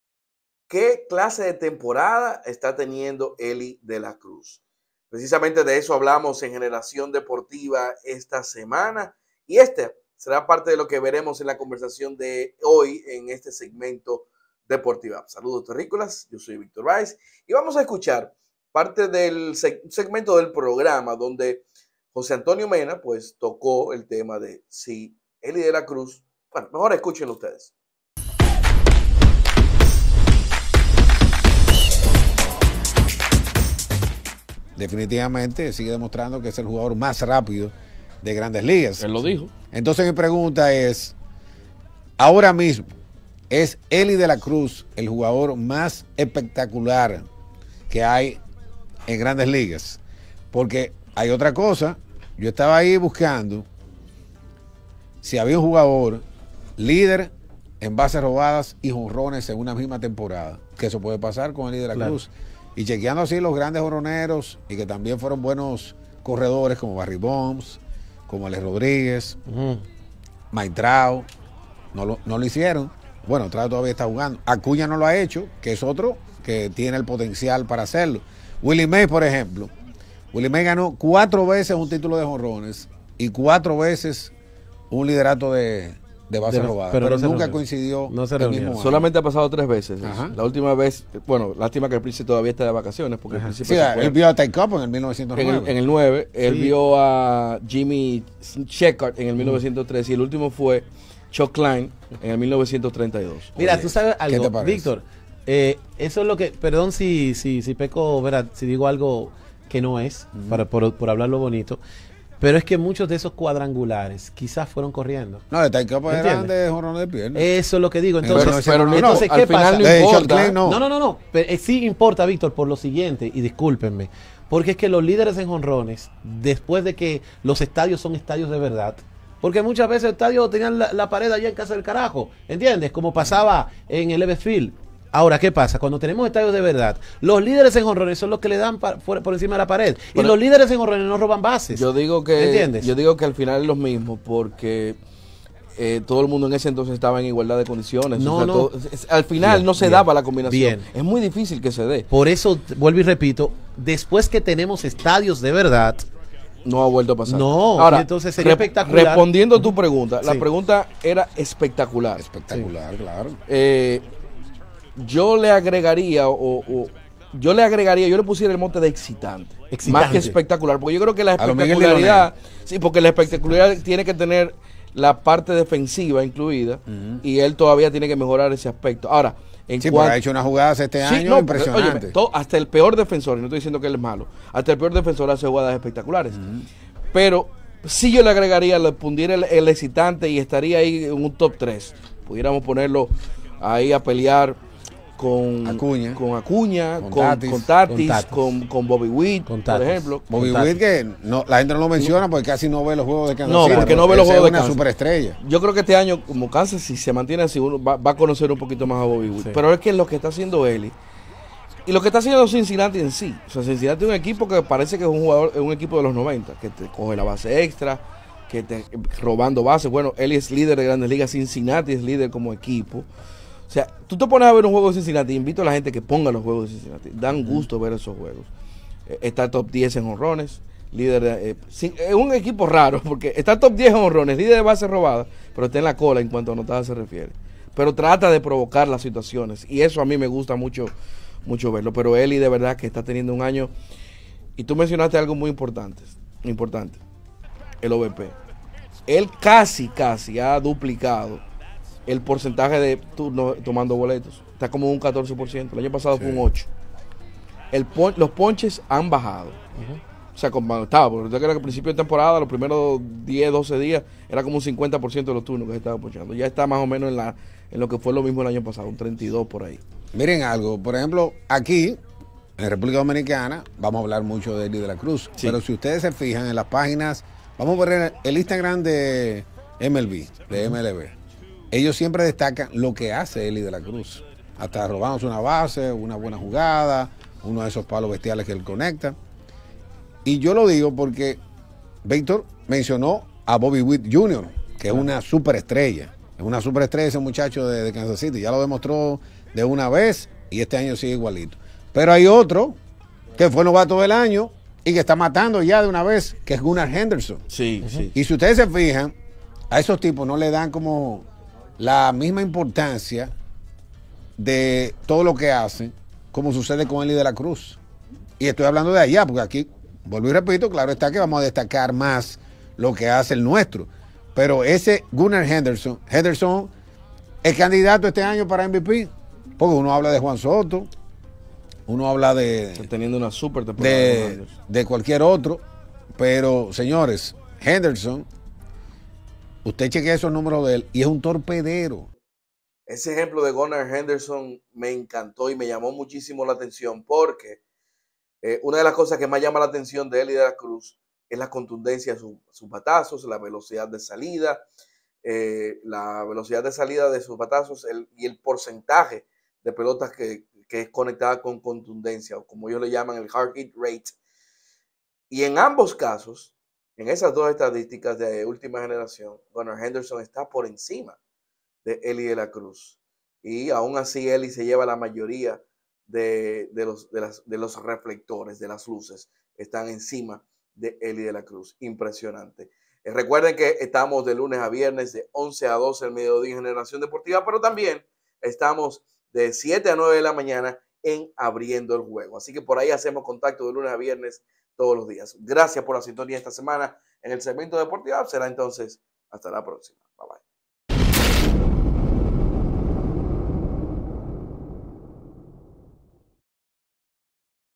Es Elly De la Cruz el más excitante jugador de Grandes Ligas?
Es Elly De la Cruz el más espectacular y excitante jugador de las Grandes Ligas en la actualidad?

Ese fue tema de debate en Generación Deportiva